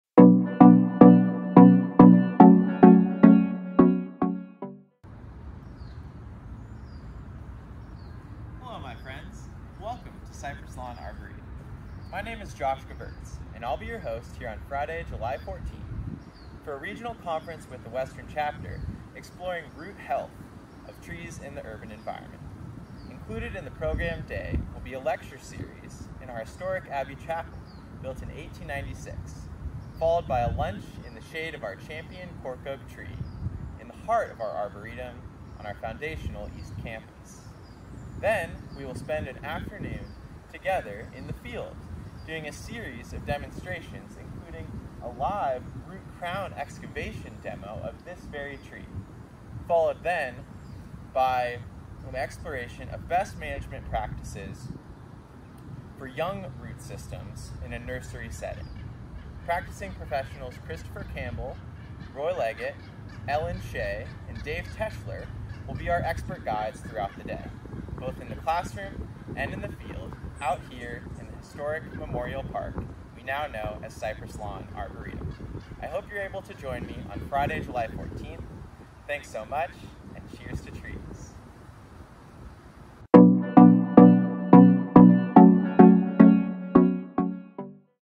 From Roots to Crown Exploring Tree Health Less Than Six Feet Under 1
This regional conference will explore root health in trees in the urban environment, with a morning lecture series and afternoon field demonstrations out on the grounds of Cypress Lawn Arboretum and Memorial Park. The subject matter for the day will focus on maintenance of healthy roots systems in mature trees, including the proper application of root crown excavations, as well as methods of cultivation of good roots in young trees in young trees in nursery settings, primarily with container-grown nursery stock. Hands-on immersive learning opportunities to understand current arboricultural methods will be taught in-person by leading practitioners in our Western Chapter region, for a day of knowledge and fun at Cypress Lawn Arboretum!